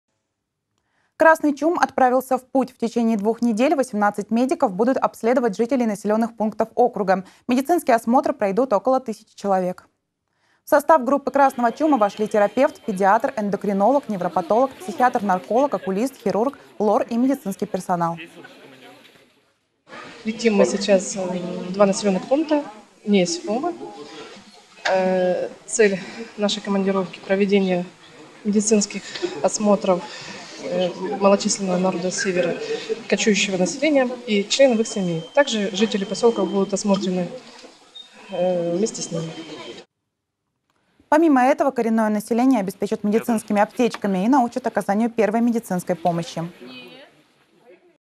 Врачи «Красного чума» отправятся в Ому и Несь
Бригада врачей поликлиники Заполярного района сегодня отправится в Ому в рамках проекта «Красный чум». Медики планируют вести прием в селе в течение недели, а затем отправятся в Несь, где проработают до 6 декабря. В составе бригады врач общей практики, хирург, невролог, окулист, лор, психиатр-нарколог, стоматолог, педиатр, эндокринолог, дерматолог и флюоротряд.